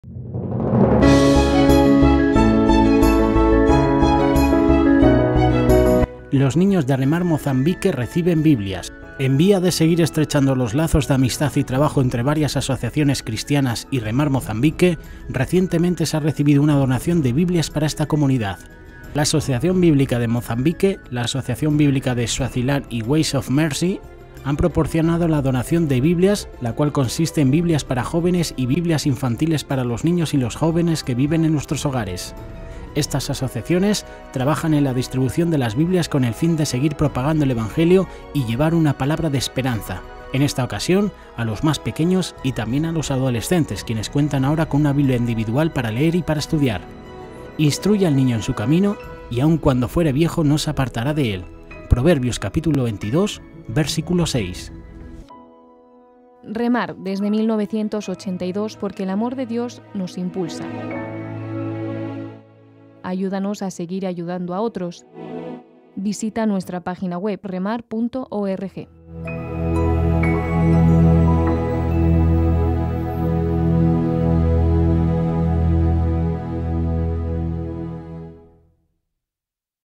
REMAR ONG - niños de Remar Mozambique reciben biblias.
En vía de seguir estrechando los lazos de amistad y trabajo entre varias asociaciones cristianas y Remar Mozambique, recientemente se ha recibido una donación de biblias para esta comunidad.